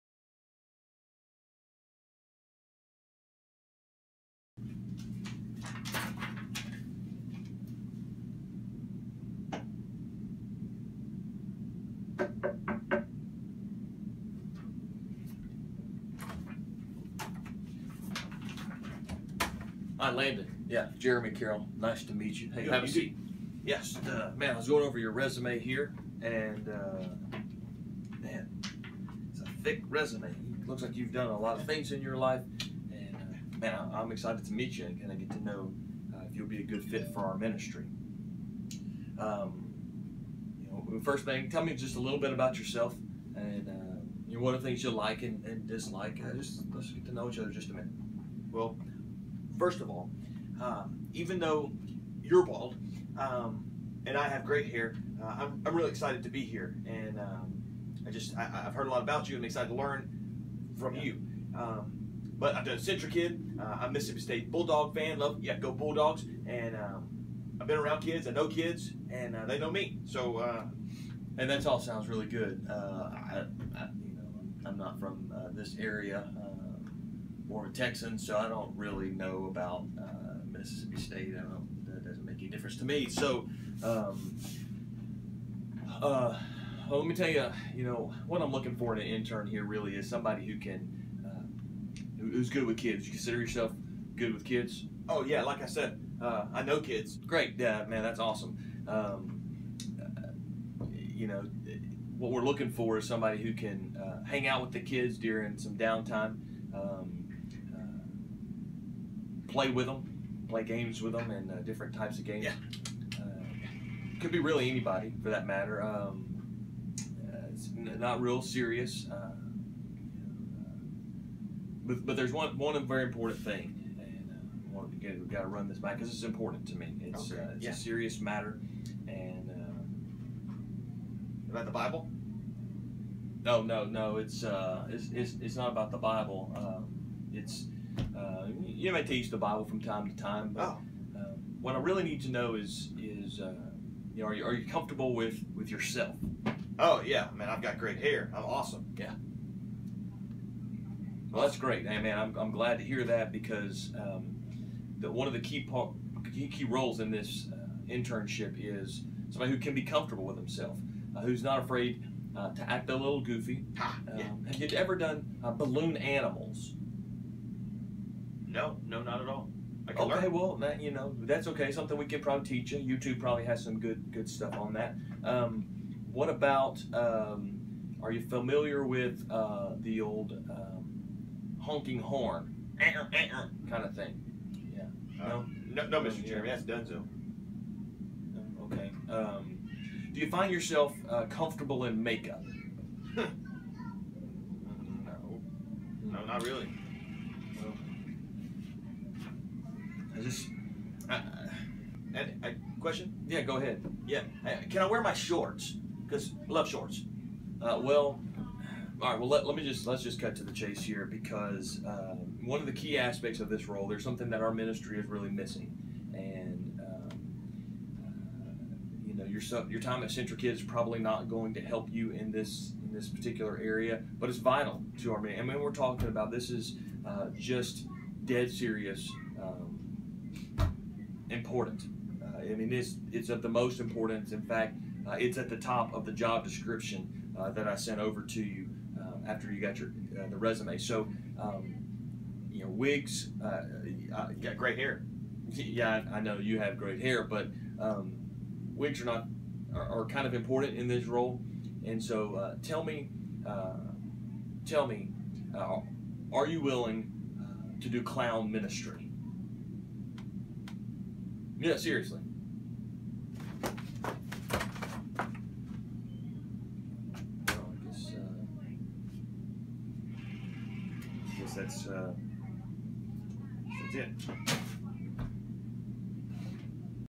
The Interview
This a parody video of an interview with a friend (Landon Tucker).